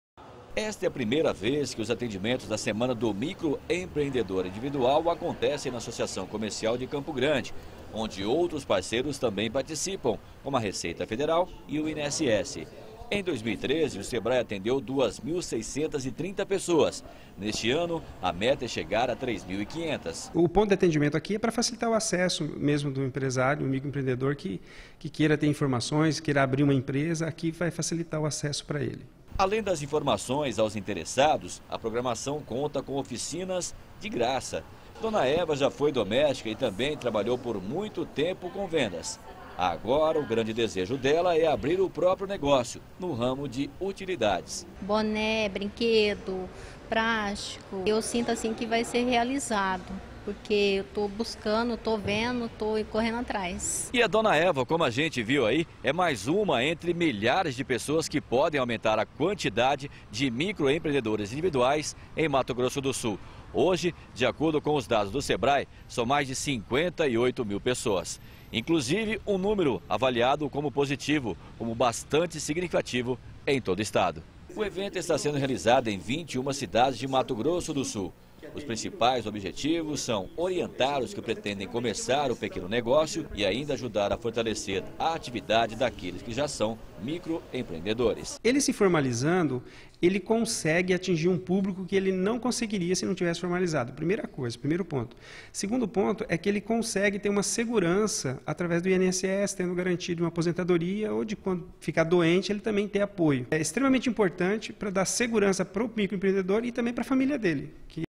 Semana do Micro Empreendedor orienta pequenos empresários
Está sendo realizada em todo o Estado a Semana do Micro Empreendedor Individual. Quem comparecer nos locais de atendimento poderá ter orientações e cursos gratuitos para formalizar o negócio. Na Capital, o atendimento está acontecendo na sede do Sebrae e também da Associação comercial.